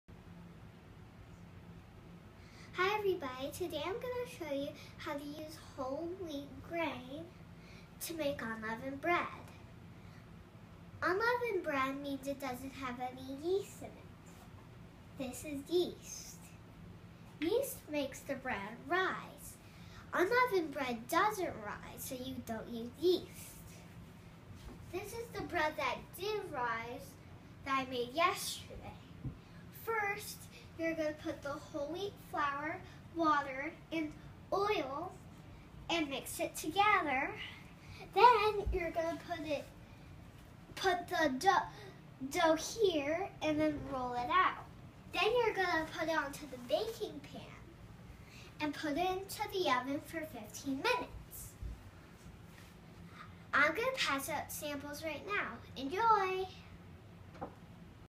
Wheat Project Kindergarten
Unleavened bread matzo crackers and matzo ball nutrition school project on wheat.